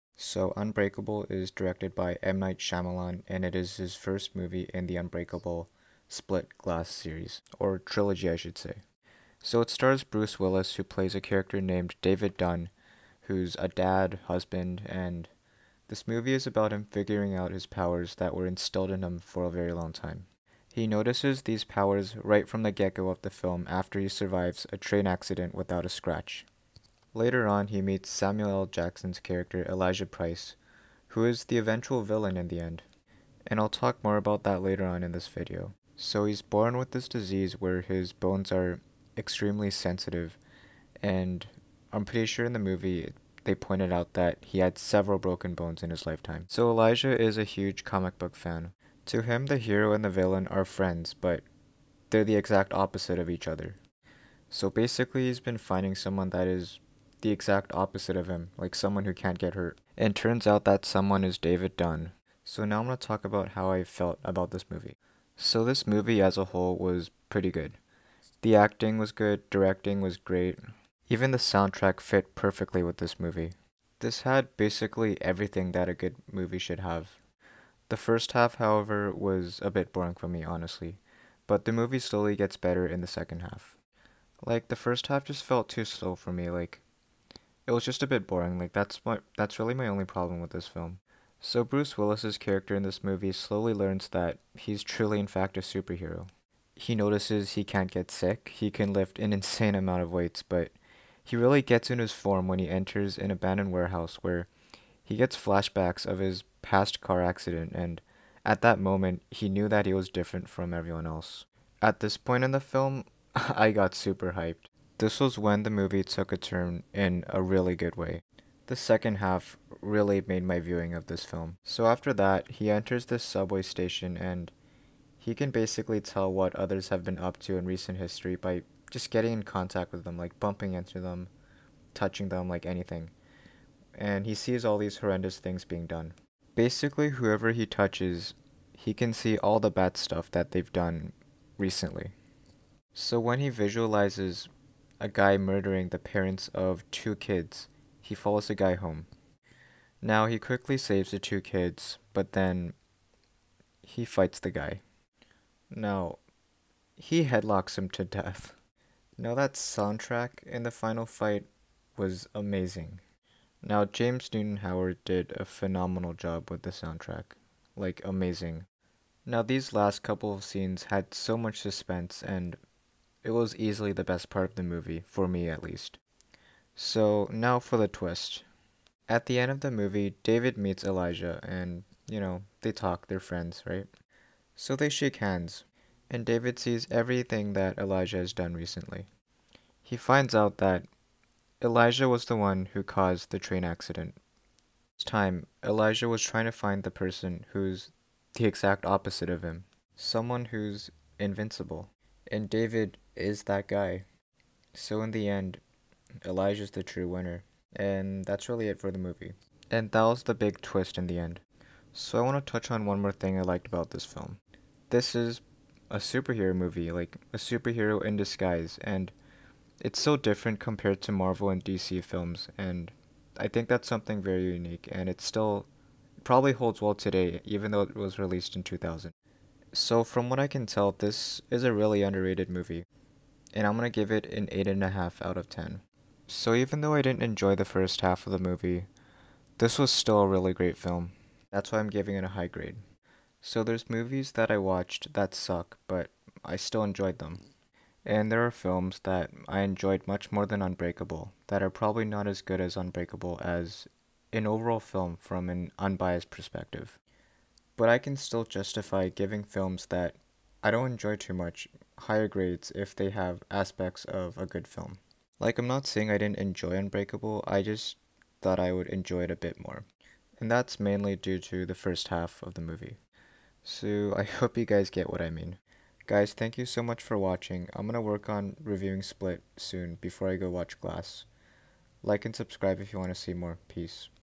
Unbreakable (2000) - Movie Review (Spoilers)
Today I review Unbreakable starring Bruce Willis, Samuel L. Jackson, Spencer Treat Clark, Robin Wright, Eamonn Walker, Chance Kelly, and Charlayne Woodard. Directed by M. Night Shyamalan.